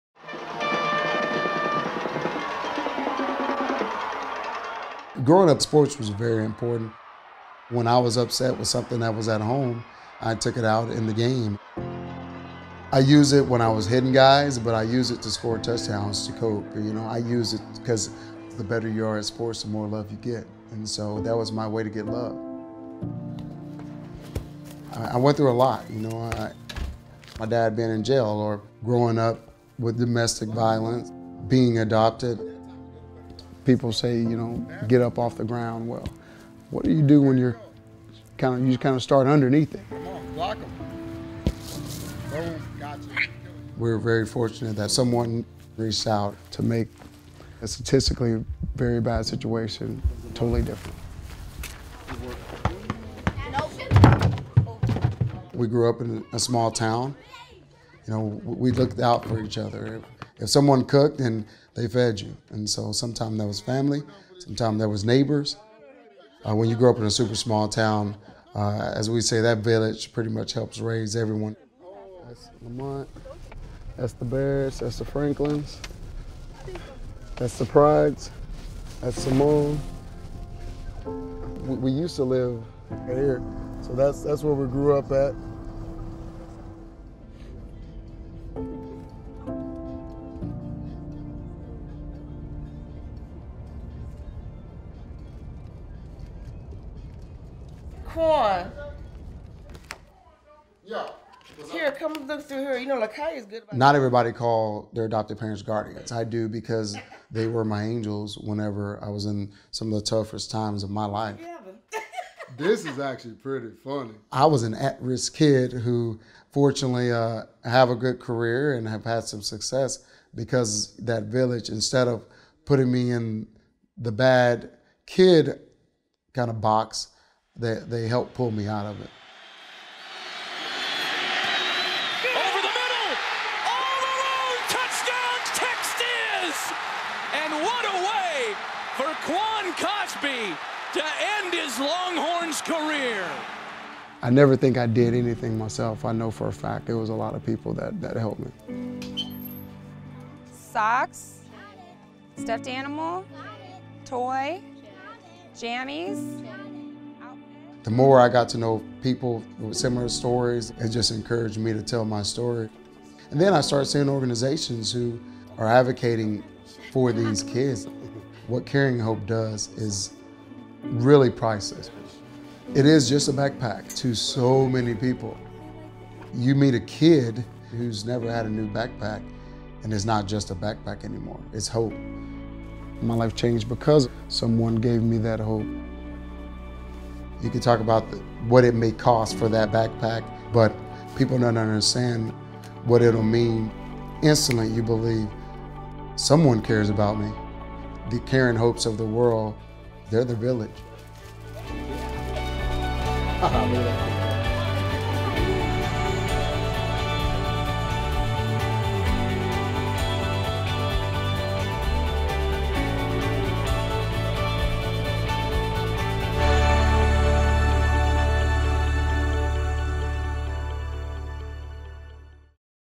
Quan Cosby & Carrying Hope (captions)
Former Texas Longhorns star and NFL player Quan Cosby shares his personal experience with adoption and how Carrying Hope impacts children who are going through similar experiences. We are so grateful to Quan and his dedication to helping foster children in Central Texas!